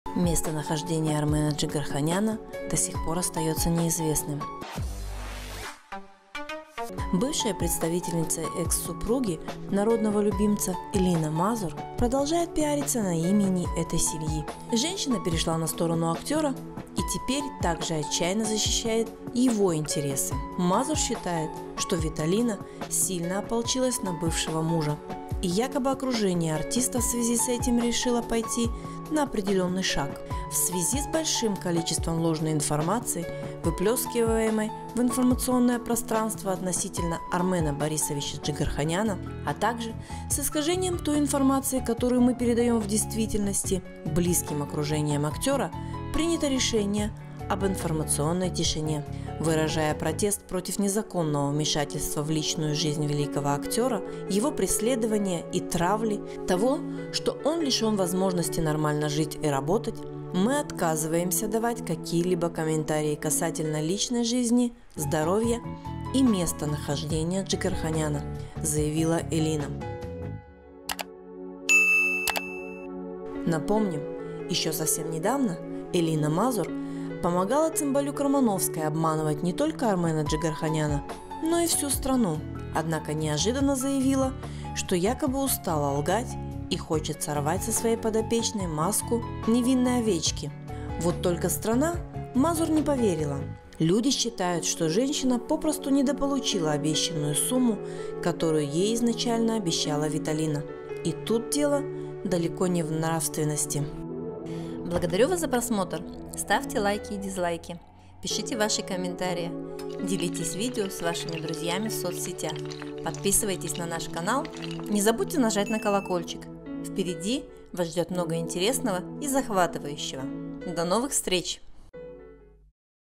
МЕСТОНАХОЖДЕНИЕ АРМЕНА ДЖИГАРХАНЯНА ОСТАЕТСЯ НЕИЗВЕСТНЫМ!
Бывшая представительница экс-супруги народного любимца Джигарханяна Элина Мазур продолжает пиариться на имени этой семьи. Женщина перешла на сторону актера и теперь так же отчаянно защищает  его интересы.